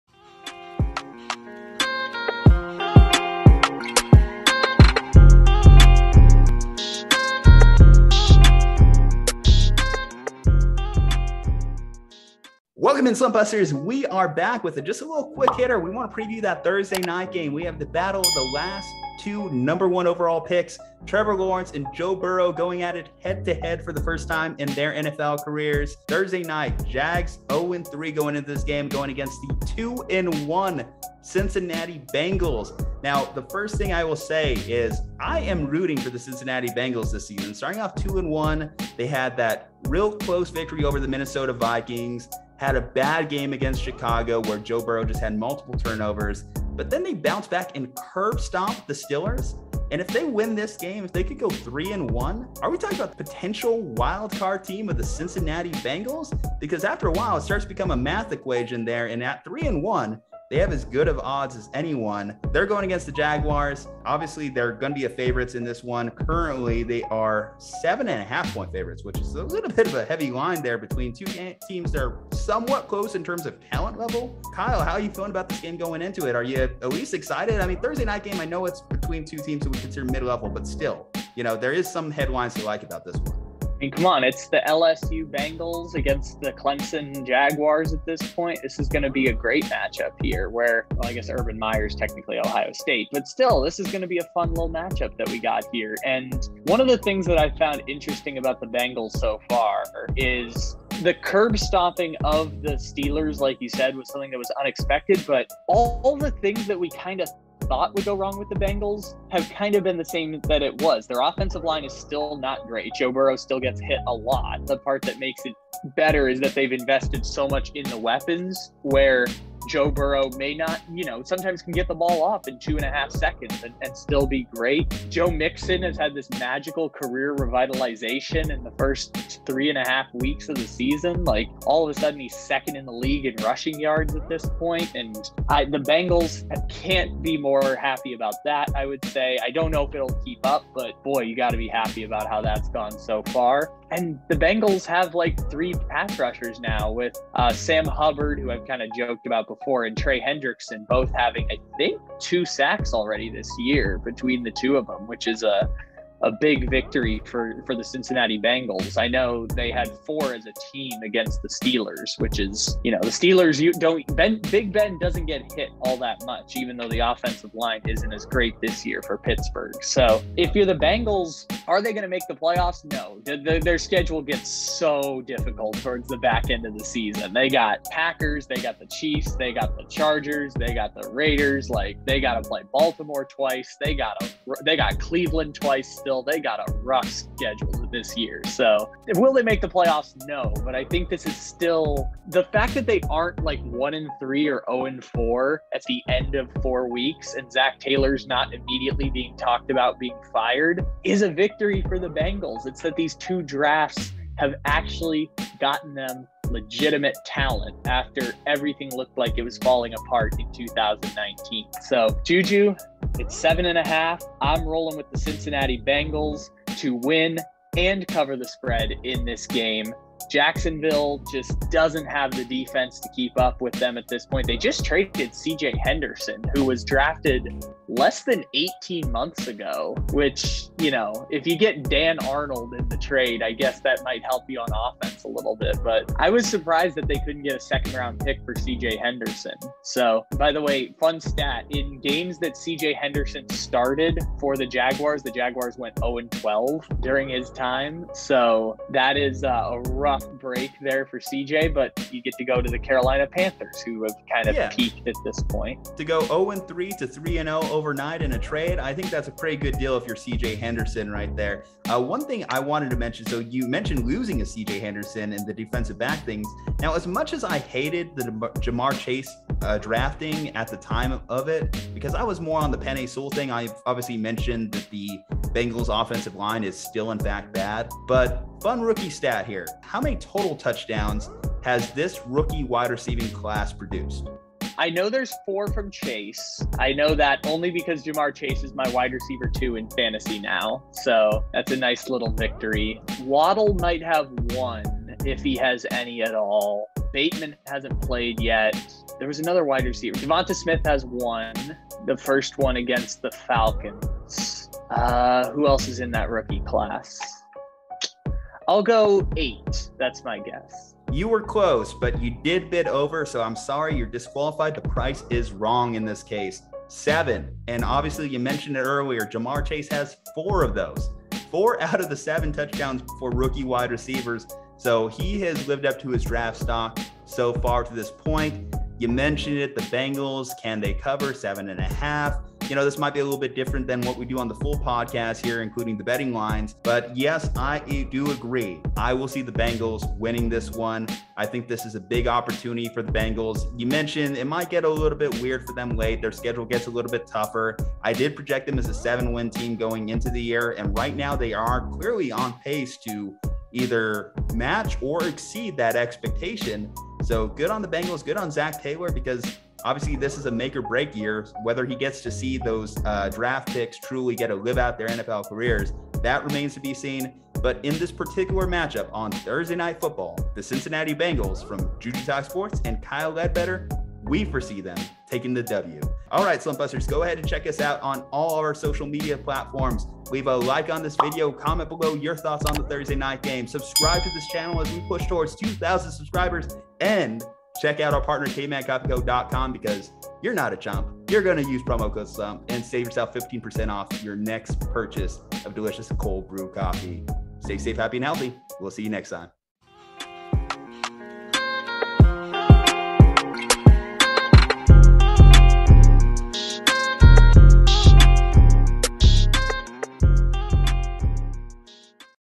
NFL Week 4: Jacksonville Jaguars @ Cincinnati Bengals Preview & Predictions | The Slumpbuster
Hey Slumpbuster! In this quick hitter we preview the Thursday Night Football showdown between the Jacksonville Jaguars and Cincinnati Bengals. For the second time in NFL history, the consecutive number one overall drafted quarterbacks will compete against each other, Joe Burrow (2020) vs Trevor Lawrence (2021). The Bengals are coming off an excellent performance against their division rival Steelers. They applied pressure to Ben Roethlisberger and rookie Ja'Marr Chase has continued to shine. Lawrence and the Jaguars have seen very little success at any point this season. Their biggest claim so far on the young year is a competitive first half performance against a red hot Cardinals team. The Bengals have an excellent opportunity to position themselves in the playoff picture while Jacksonville is looking for its first win in 2021. Who will emerge victorious Thursday night?

Recorded :  Wednesday, September 29, 2021

Host:
Julian "Ju-Ju" Martinez
Ig: @jujutalkssports | Twitter: @jujutalkssports

Kyle Ledbetter
Take It Easy Podcast
Ig: @kyle.ledbetter619 | Twitter: @CSM_MemeInsta